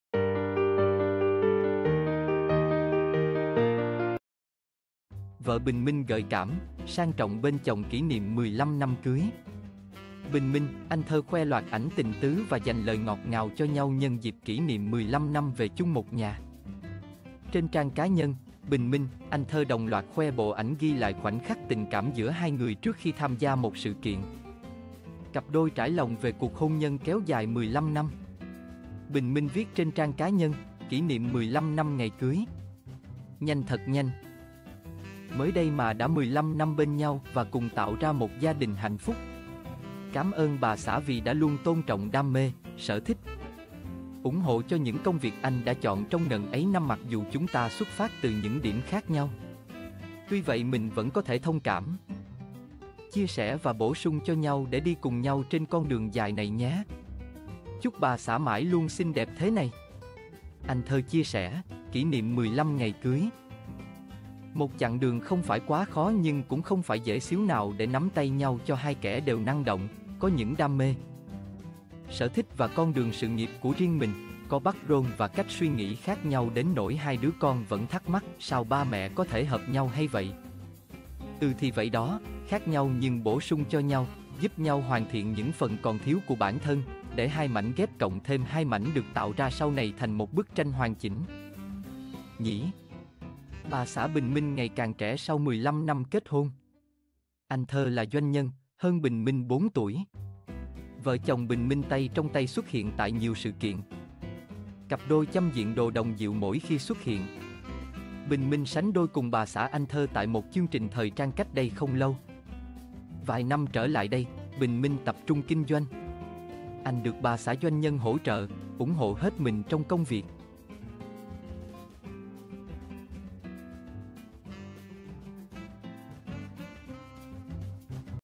Vợ Bình Minh gợi cảm, sang trọng bên chồng kỷ niệm 15 năm cưới
Bình Minh - Anh Thơ khoe loạt ảnh tình tứ và dành lời ngọt ngào cho nhau nhân dịp kỷ niệm 15 năm về chung một nhà.
Trên trang cá nhân, Bình Minh - Anh Thơ đồng loạt khoe bộ ảnh ghi lại khoảnh khắc tình cảm giữa hai người trước khi tham gia một sự kiện.
Cặp đôi trải lòng về cuộc hôn nhân kéo dài 15 năm.
Bình Minh viết trên trang cá nhân: "Kỷ niệm 15 năm ngày cưới. Nhanh thật nhanh, mới đây mà đã 15 năm bên nhau và cùng tạo ra một gia đình hạnh phúc. Cám ơn bà xã vì đã luôn tôn trọng đam mê, sở thích, ủng hộ cho những công việc anh đã chọn trong ngần ấy năm mặc dù chúng ta xuất phát từ những điểm khác nhau. Tuy vậy mình vẫn có thể thông cảm, chia sẻ và bổ sung cho nhau để đi cùng nhau trên con đường dài này nhé. Chúc bà xã mãi luôn xinh đẹp thế này".
Anh Thơ chia sẻ: “Kỷ niệm 15 ngày cưới, một chặng đường không phải quá khó nhưng cũng không phải dễ xíu nào để nắm tay nhau cho hai kẻ đều năng động, có những đam mê, sở thích và con đường sự nghiệp của riêng mình, có background và cách suy nghĩ khác nhau đến nỗi hai đứa con vẫn thắc mắc “sao ba mẹ có thể hợp nhau hay vậy”. Ừ thì vậy đó, khác nhau nhưng bổ sung cho nhau, giúp nhau hoàn thiện những phần còn thiếu của bản thân, để hai mảnh ghép cộng thêm 2 mảnh được tạo ra sau này thành một bức tranh hoàn chỉnh, nhỉ”.
Bà xã Bình Minh ngày càng trẻ sau 15 năm kết hôn.
Anh Thơ là doanh nhân, hơn Bình Minh 4 tuổi.
Vợ chồng Bình Minh tay trong tay xuất hiện tại nhiều sự kiện.
Cặp đôi chăm diện đồ đồng diệu mỗi khi xuất hiện.
Bình Minh sánh đôi cùng bà xã Anh Thơ tại một chương trình thời trang cách đây không lâu.
Vài năm trở lại đây, Bình Minh tập trung kinh doanh. Anh được bà xã doanh nhân hỗ trợ, ủng hộ hết mình trong công việc.